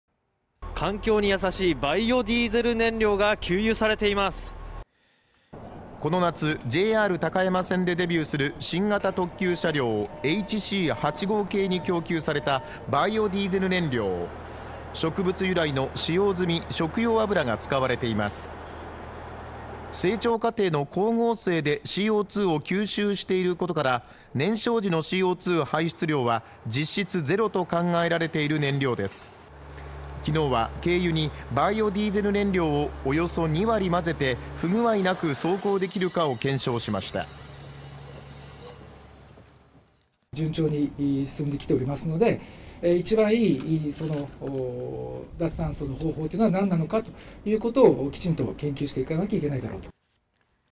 ＪＲ東海の新型特急「HC85系」 燃料に使用済み食用油 CO2排出実質ゼロ？ 不具合なく走れるか検証 (22/02/09 20:43)
「脱炭素」に向けたＪＲ東海の検証試験。食用油を使ったバイオディーゼル燃料の実力は。

　この夏、ＪＲ高山線でデビューする、新型特急車両「ＨＣ８５系」に供給された、バイオディーゼル燃料。

　植物由来の、使用済みの食用油が使われています。

　植物が原料なので、成長過程での光合成で、Ｃ０２を吸収していることから、燃焼時のＣＯ２排出量は、実質ゼロと考えられている燃料です。

　９日報道公開された検証試験では、軽油にバイオディーゼル燃料をおよそ２０パーセント混ぜて、不具合なく走行できるかを検証しました。

　ＪＲ東海技術開発部石原光昭チームマネージャー
　「順調に進んできている。一番いい脱炭素の方法とは何なのか、しっかり研究していかなければならない」